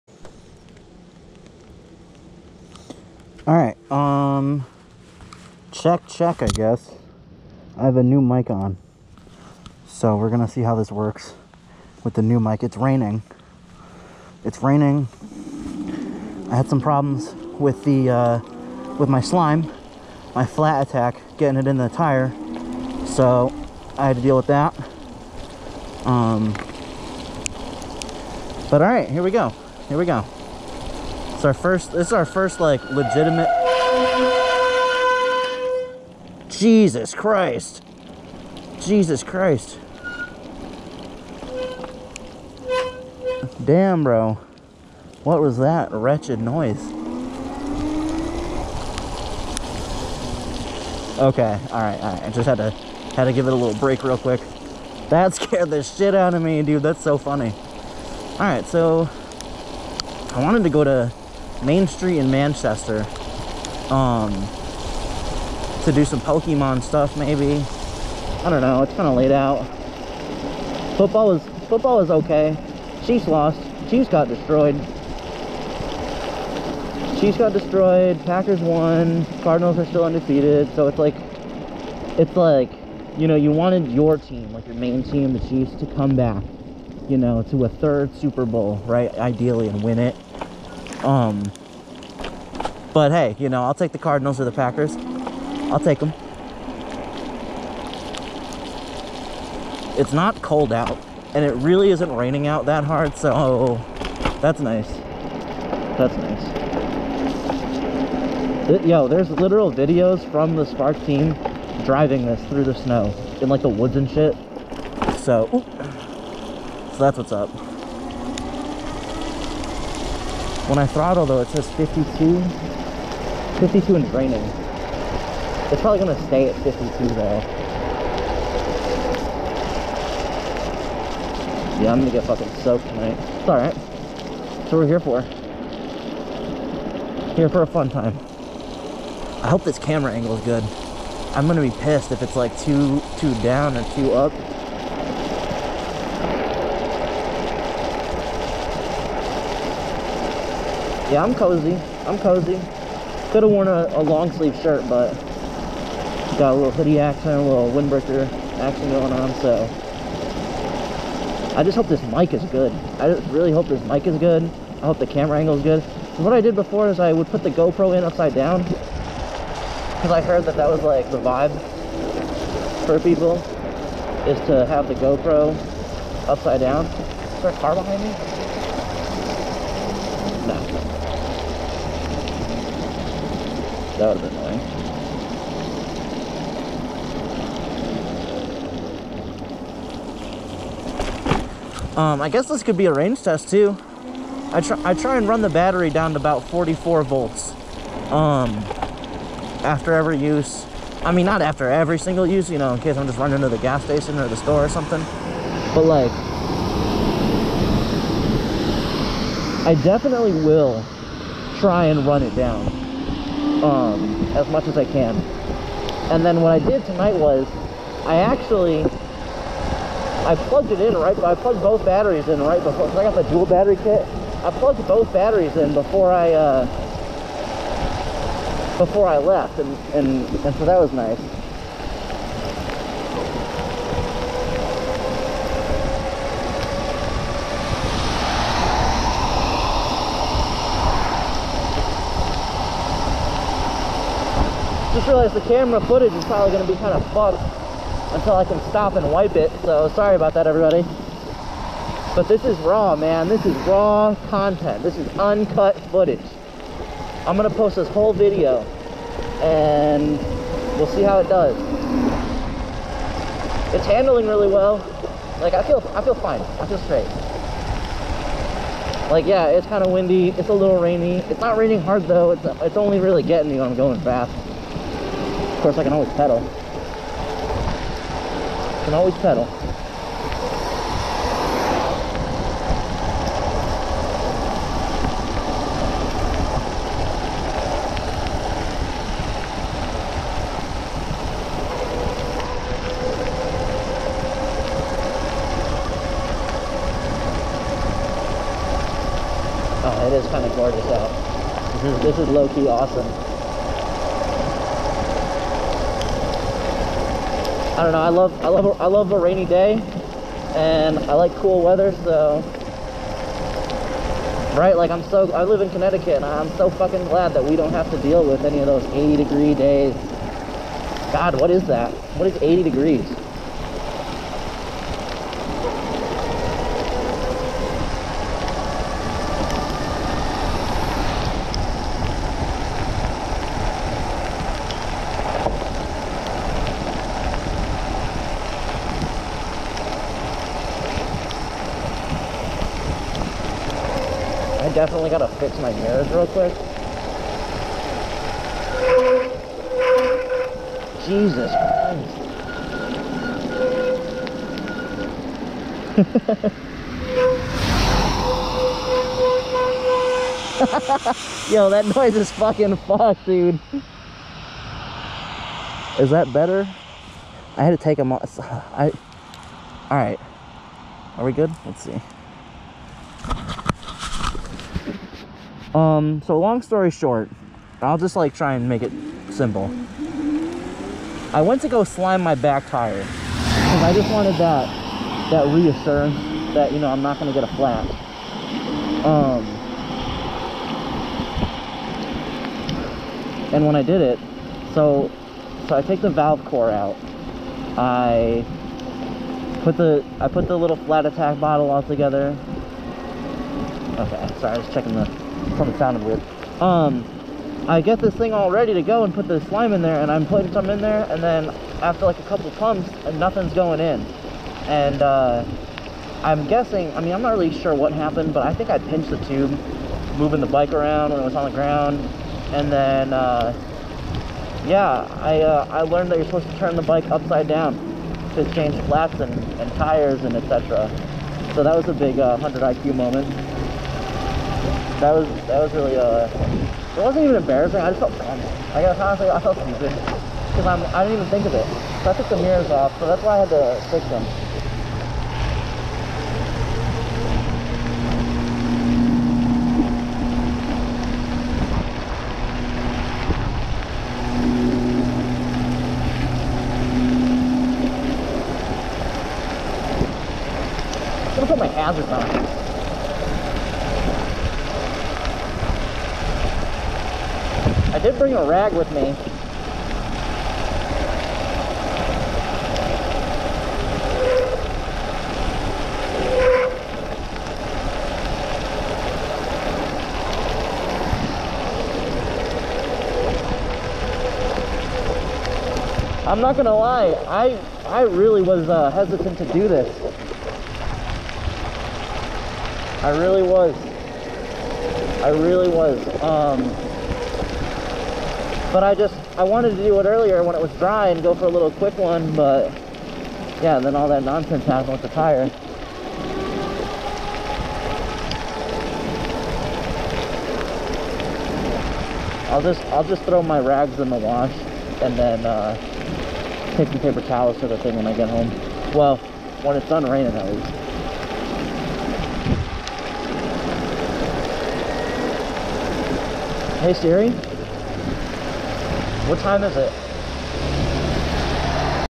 ⚡️The Spark Cycle Works Bandit⚡️ (Nighttime/light rain ride/New mic test issues with Flat attack!
Hey y’all ! Tonight I wanted to do a ride in the rain and test out my new mic which works really well, unfortunately I should’ve stuck with keeping the go pro mounted upside down on the chest mount.

the whole video is aimed down 😭 we have a chin mount coming soon but tonight was also just a mic test as well.

Next video will have a better angle I got yall ! 😅